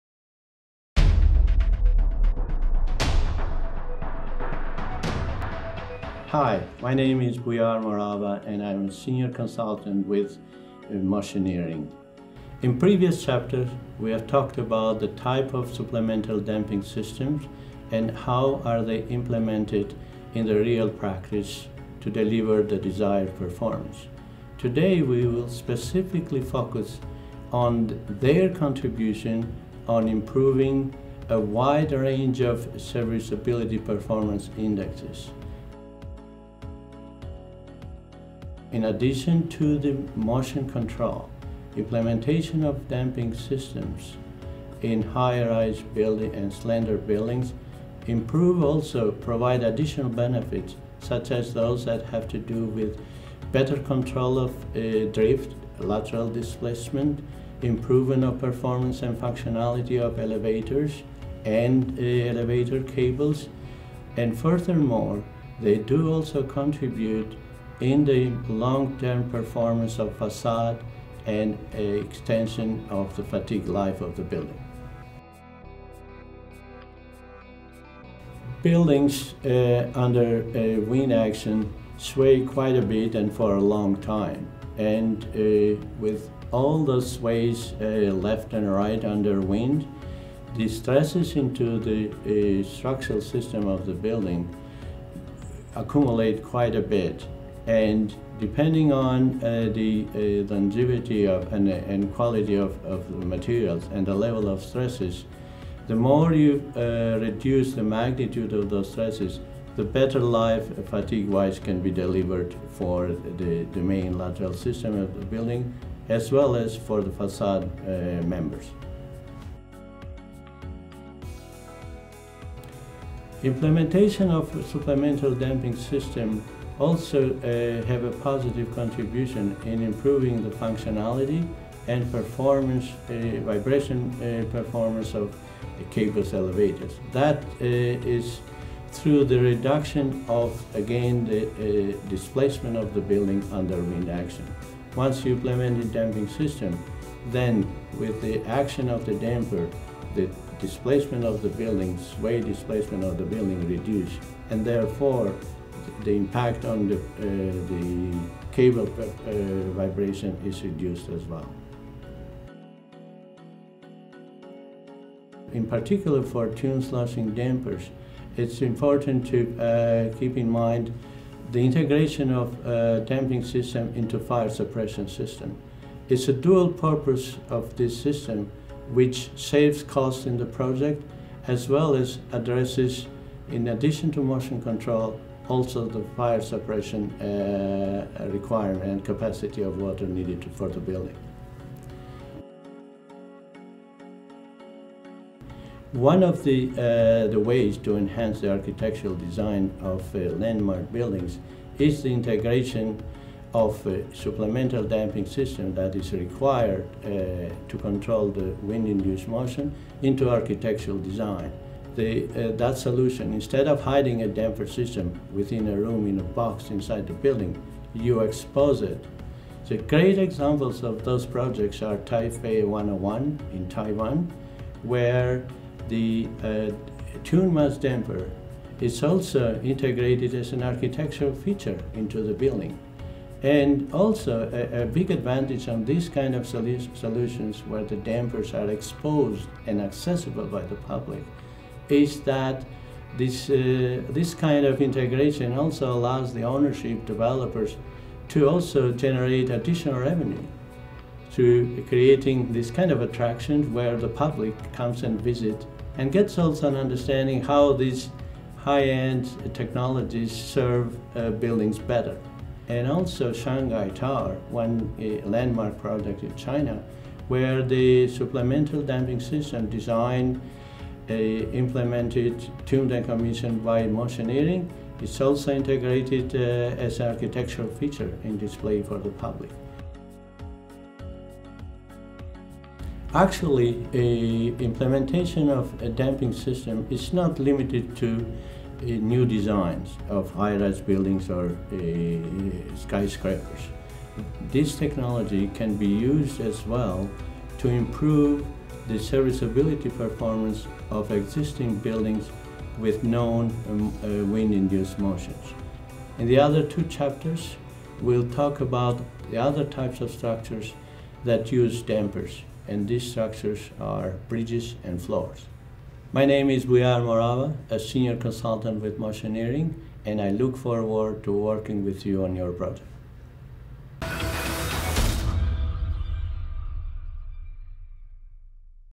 How Dampers and Tuned Mass Dampers Enhance Building Performance
Damping Explained is a video series that breaks down the ins and outs of structural motion control systems in the built environment. When a structure is subjected to external forces — wind, pedestrian movement or seismic activity, for example — it vibrates or sways. In tall buildings and on large bridges, such movement can make people uncomfortable. In other structures, it can accelerate material fatigue or other failures. Damping is one way to reduce this motion.

In this video, Bujar Morava, Senior Consultant at Motioneering, explains how dampers and tuned mass dampers can improve building performance. Sometimes tuned mass dampers can even be integrated into the architectural design – like in Taipei 101 or Shanghai Tower.